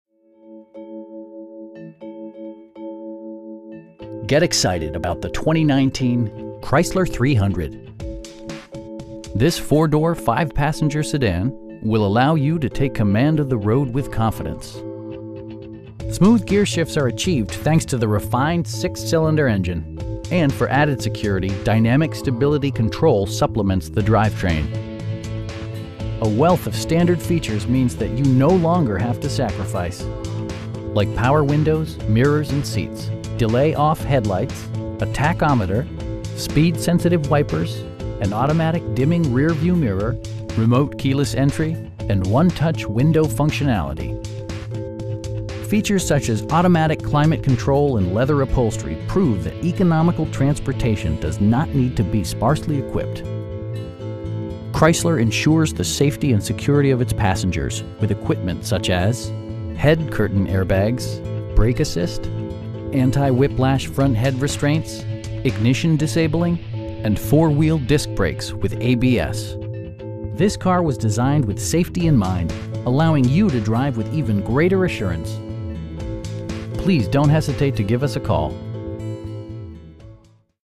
2019 Chrysler 300 TOURING L in Chesapeake, VA 23320
Get excited about the 2019 Chrysler 300. This 4 door, 5 passenger sedan will allow you to take command of the road with confidence! Smooth gearshifts are achieved thanks to the refined 6 cylinder engine, and for added security, dynamic Stability Control supplements the drivetrain. A wealth of standard features means that you no longer have to sacrifice. Like power windows, mirrors, and seats, delay-off headlights, a tachometer, speed sensitive wipers, an automatic dimming rear-view mirror, remote keyless entry, cruise control, and 1-touch window functionality. Features such as automatic climate control and leather upholstery prove that economical transportation does not need to be sparsely equipped. For drivers who enjoy the natural environment, a power moon roof allows an infusion of fresh air. Chrysler ensures the safety and security of its passengers with equipment such as: dual front impact airbags with occupant sensing airbag, head curtain airbags, traction control, brake assist, anti-whiplash front head restraints, ignition disabling, an emergency communication system, and 4 wheel disc brakes with ABS. This car was designed with safety in mind, allowing you to drive with even greater assurance. We pride ourselves in the quality that we offer on all of our vehicles. Please don't hesitate to give us a call.